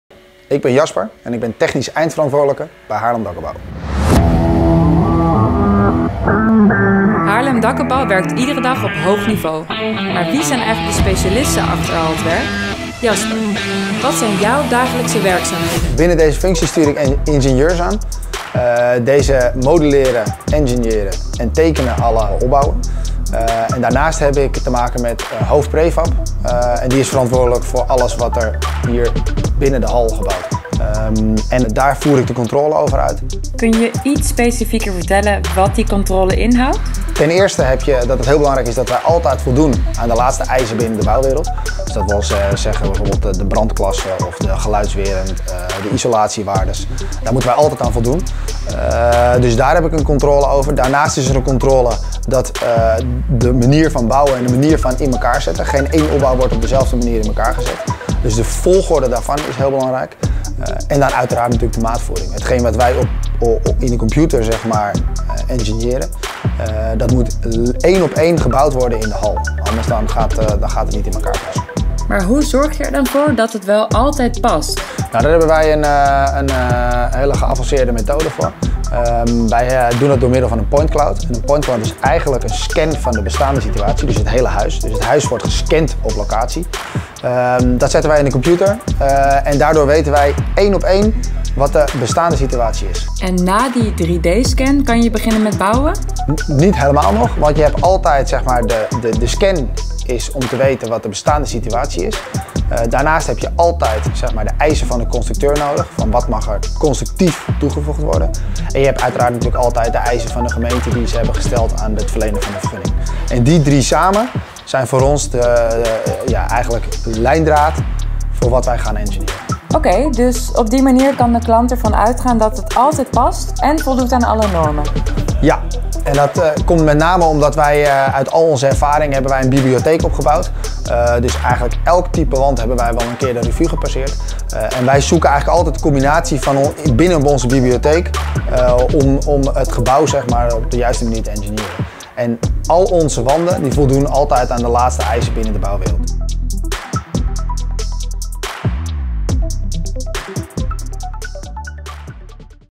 Jasper - Haarlem Dakopbouw
Haarlem Dakopbouw werkt iedere dag op hoog niveau. Een team van specialisten werkt samen aan dakopbouwen van de hoogste kwaliteit. Maar wie zijn eigenlijk deze specialisten achter het werk? Wat gebeurt er bijvoorbeeld voorafgaand aan het plaatsen van de dakopbouw allemaal? En hoe wordt er samengewerkt om zo goed en efficiënt mogelijk te kunnen bouwen? In deze serie maak je kennis met een aantal van onze specialisten. We lopen met ze mee en ze vertellen meer over hun werk. Vandaag spreken we met Jasper, mede-eigenaar en technisch eindverantwoordelijke bij Haarlem Dakopbouw.